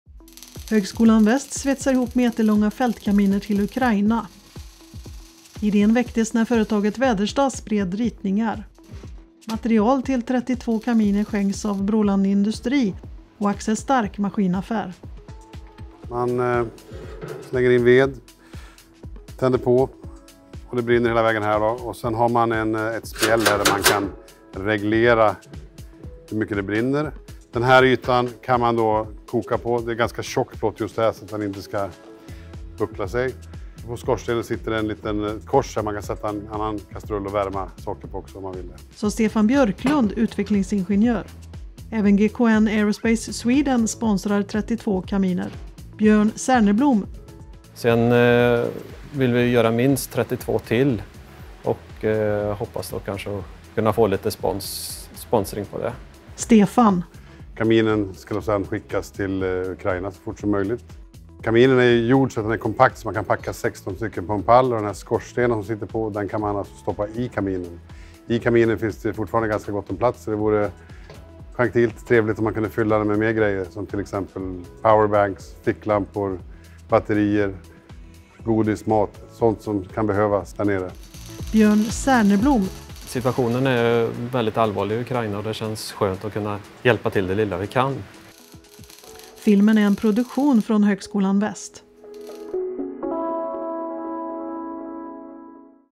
Högskolan Väst svetsar fältkaminer till Ukraina - syntolkat
Svetsutrustningen går på högtryck när högskolans forskningsingenjörer svetsar fältkaminer på Produktionstekniskt centrum. Industriföretag i regionen sponsrar materialet och i vår skickas ett 90-tal kaminer till Ukraina.
– Det känns gott att kunna hjälpa det ukrainska folket på något sätt, säger initiativtagaren Björn Särnerblom, forskningsingenjör på Högskolan Väst.